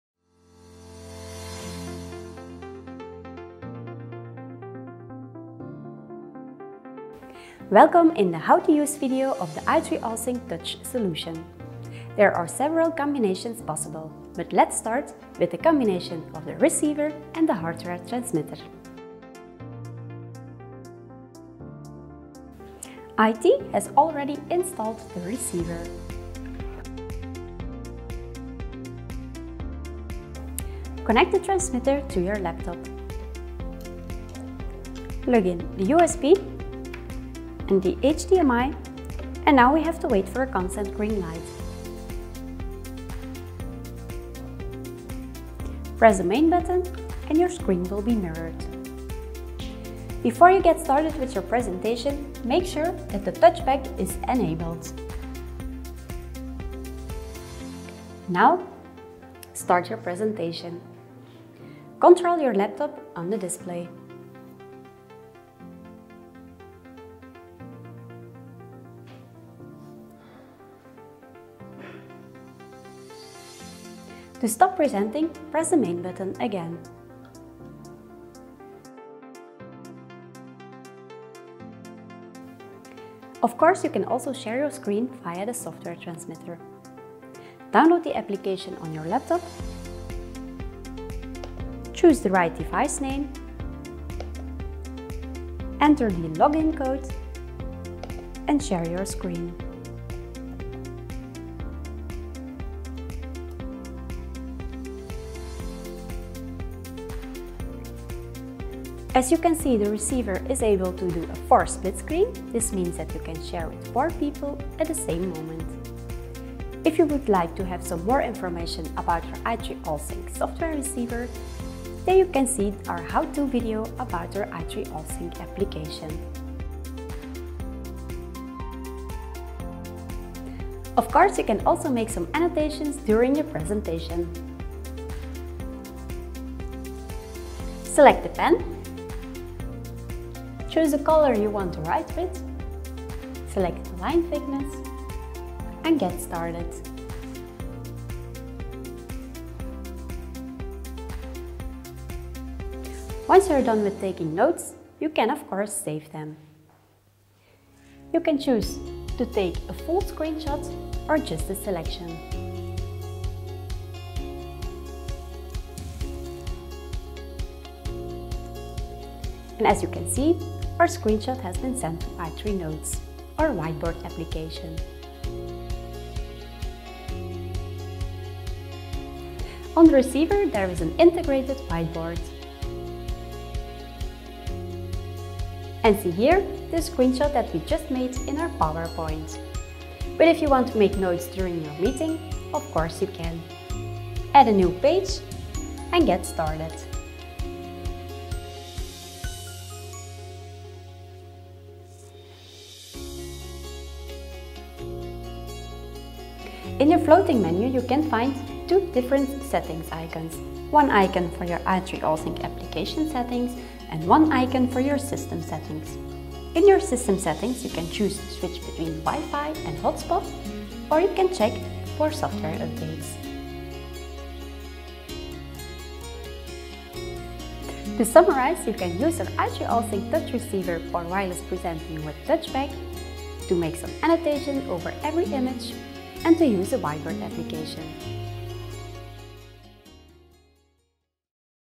i3ALLSYNC Wireless Presenting - How to Use
Tutorial on how to USE the i3ALLSYNC wireless solution. Training video.